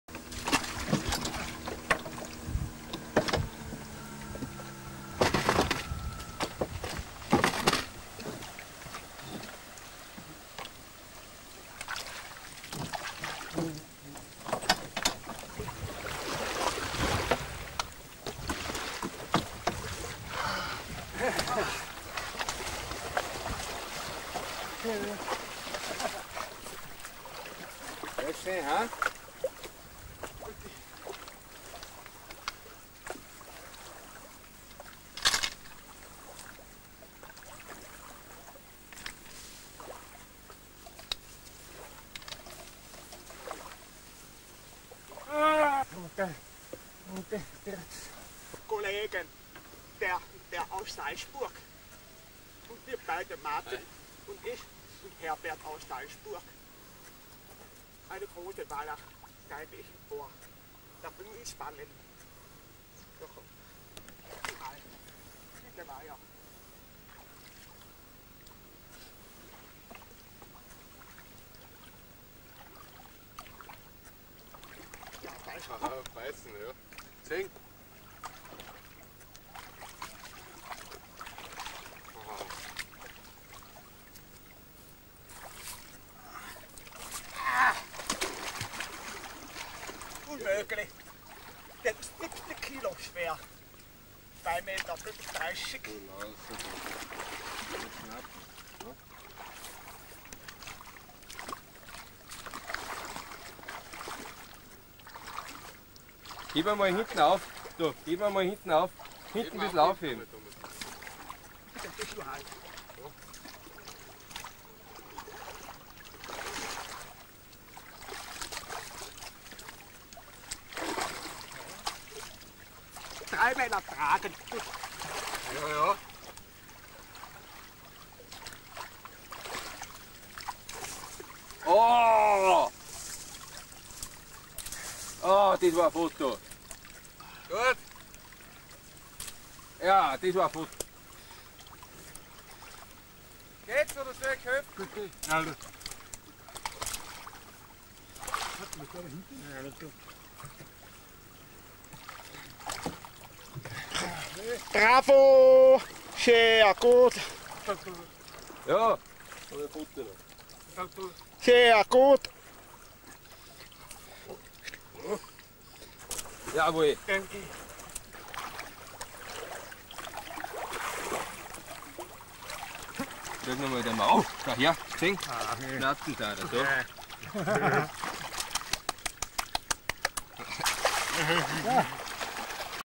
Welsangeln in Kasachtan 1996
235 cm langer Riesenwels 76 kg schwer